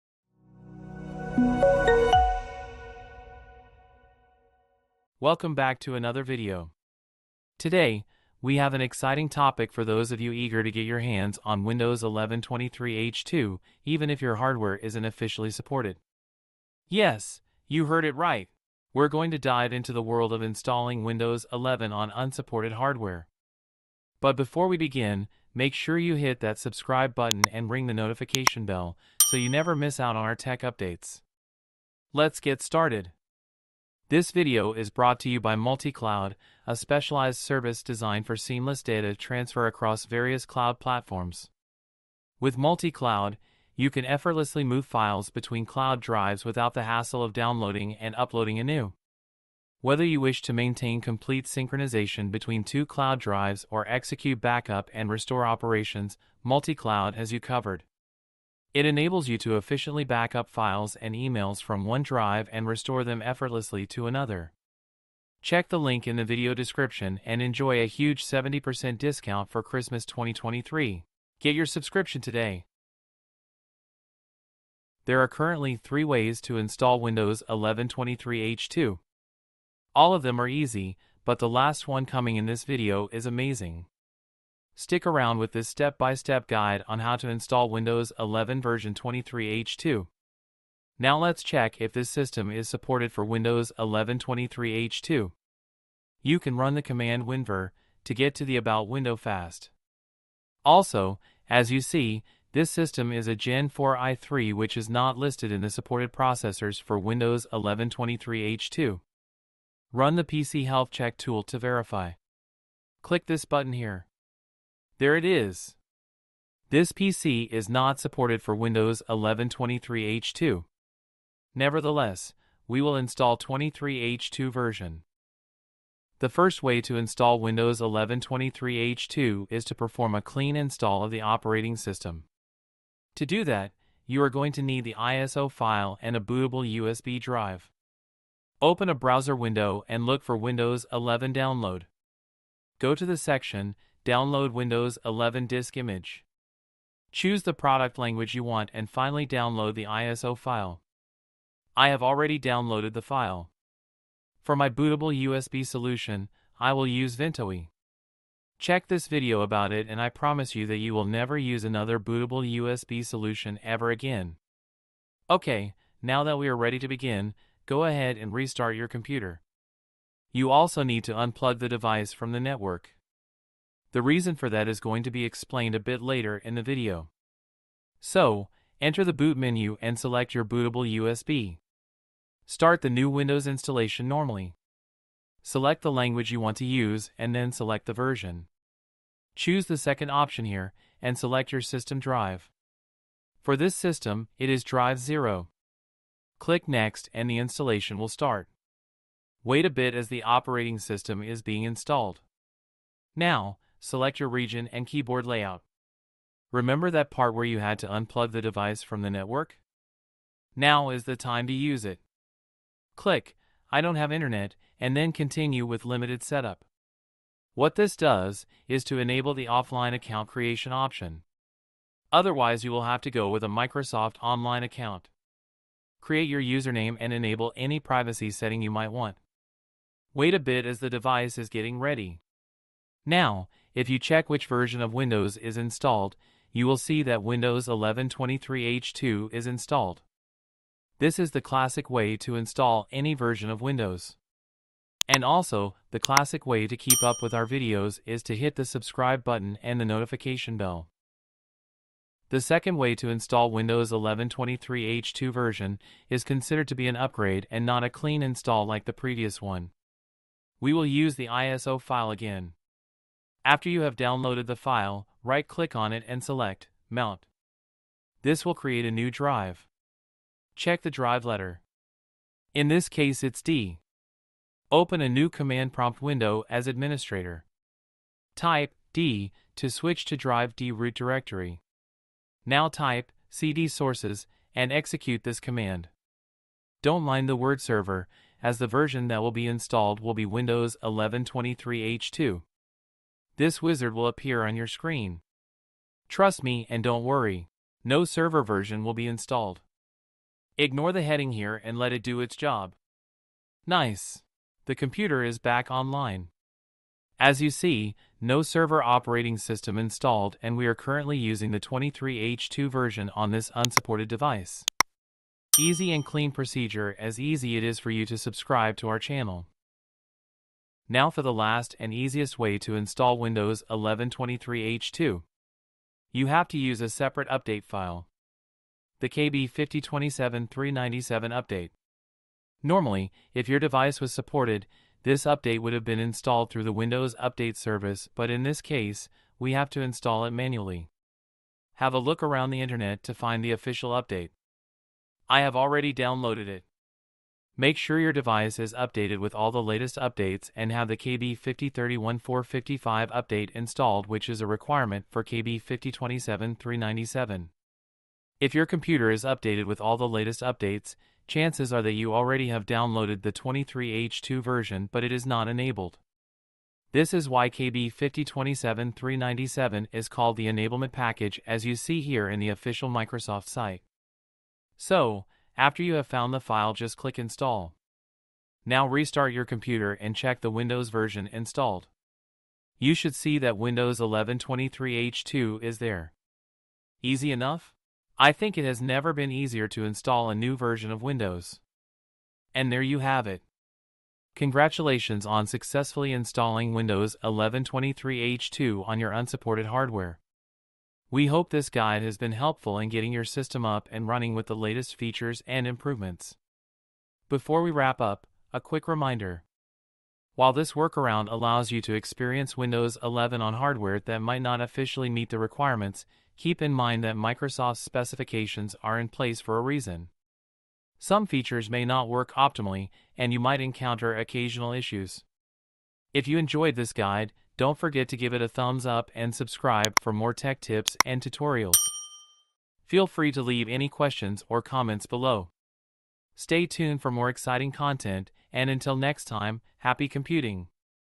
Installing Windows 11 23H2 on Unsupported Hardware: A Step-by-Step Guide
Today, we have an exciting topic for those of you eager to get your hands on Windows 11 23H2, even if your hardware isn't officially supported.
Yes, you heard it right! We're going to dive into the world of installing Windows 11 on unsupported hardware.
If you've been eager to try out the latest Windows version but concerned about compatibility, look no further
In this step-by-step guide on how to install Windows 11 23H2 on unsupported hardware, we will walk you through the entire process, ensuring a successful installation without the need for specialized hardware.
Our carefully crafted instructions will enable you to enjoy all the exciting features and enhancements of Windows 11 23H2, even on unsupported devices. In this video, we take you through each crucial step, including downloading the necessary ISO file, preparing your hardware, and bypassing compatibility checks.
We will also provide valuable tips to minimize them, ensuring a smooth upgrade experience. Whether you're a tech enthusiast or simply looking to explore the capabilities of Windows 11 23H2 on your existing hardware, this guide is designed to assist you every step of the way. Join us now and embark on this exciting journey of harnessing the power of Windows 11, regardless of hardware compatibility limitations.

Key Moments.
00:00 Intro.
01:14 Simple install from usb or iso file.
04:11 Upgrade from iso file.
05:20 Use enablement package update.
06:31 Outro.

Do you like this this channel? Help us create more and better videos for you